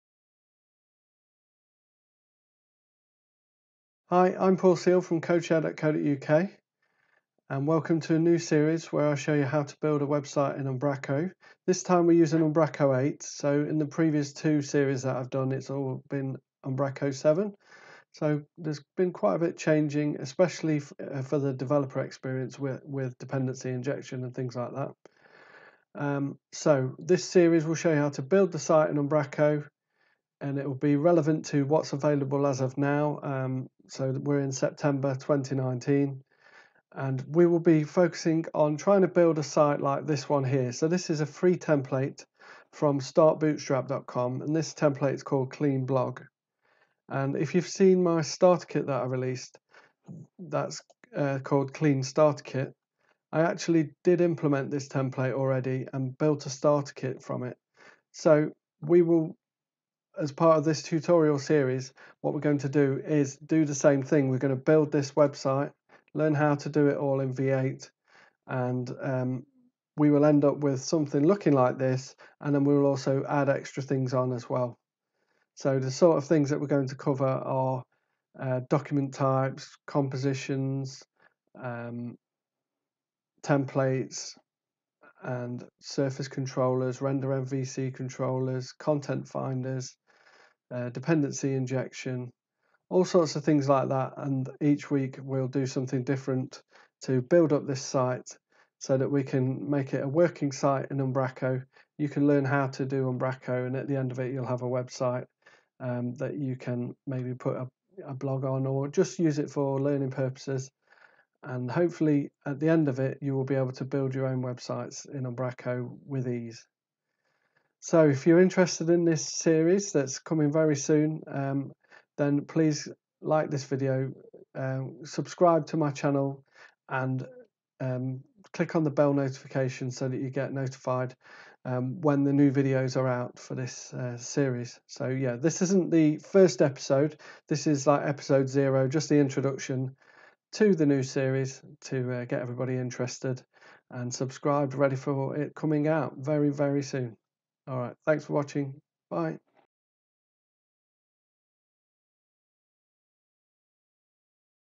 How to build a website with Umbraco v8 - Introduction
This is the introduction video for my new series on how to build a website with Umbraco v8